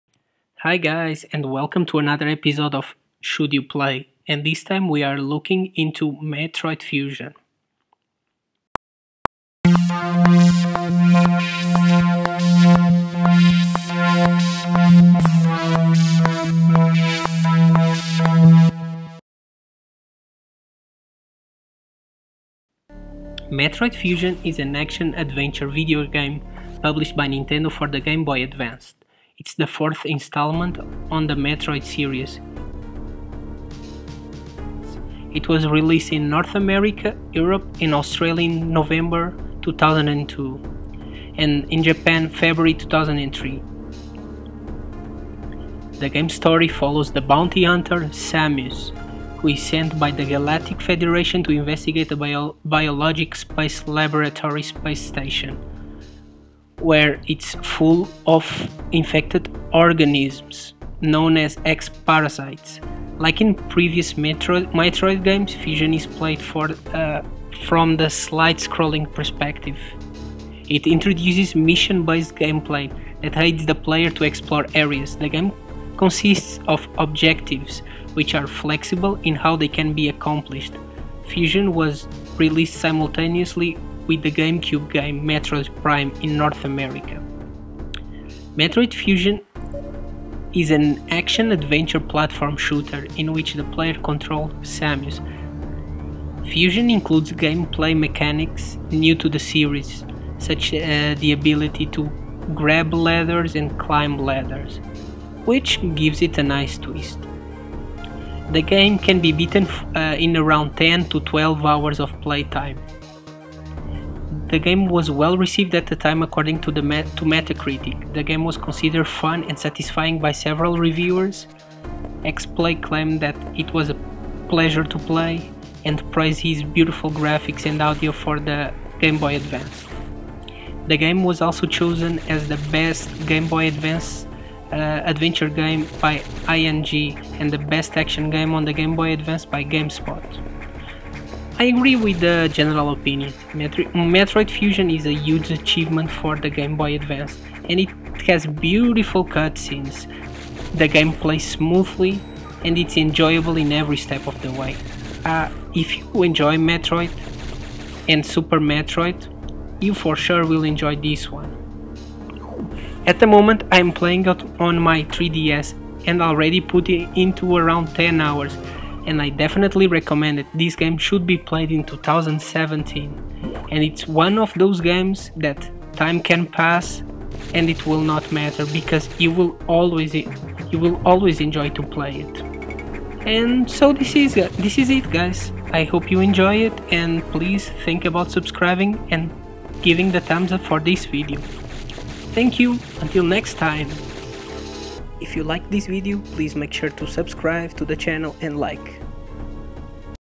Should You Play Metroid Fusion in 2017?
This time on the series "Should you play" I will be checking on Metroid Fusion, released for the gameboy advance.